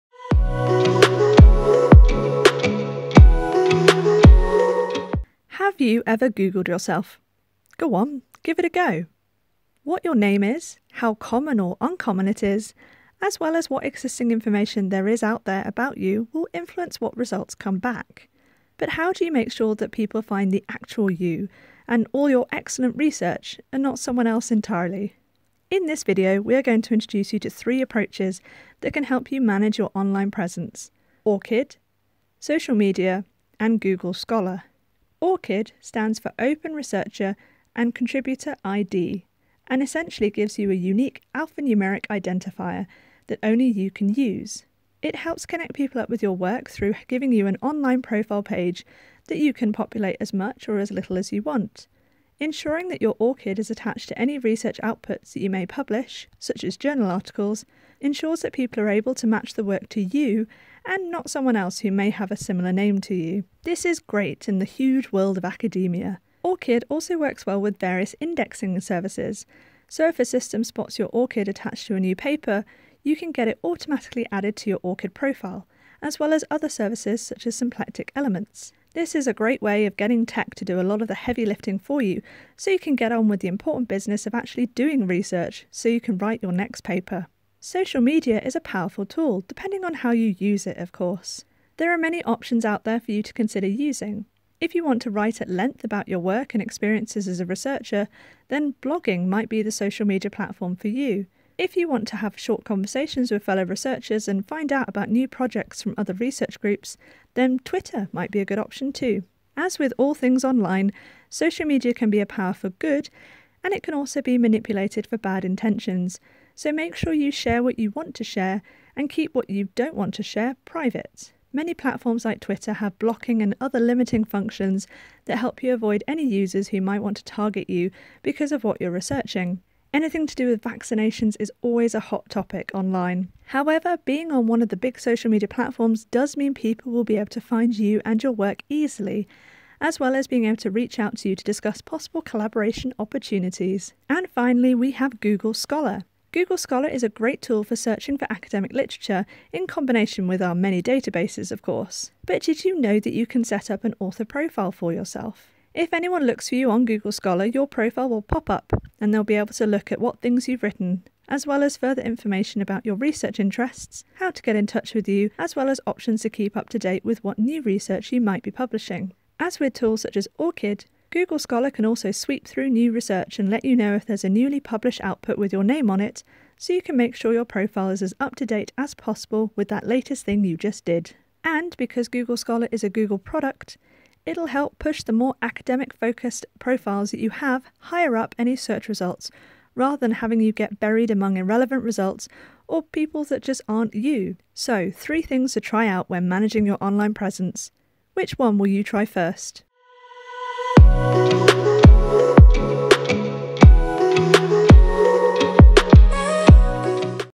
Managing Your Online Presence
Making sure that people find you and your research when they look for you online is a useful thing but how do you ensure that this happens? This video will take you through three tools you can use: ORCID, social media, and Google Scholar.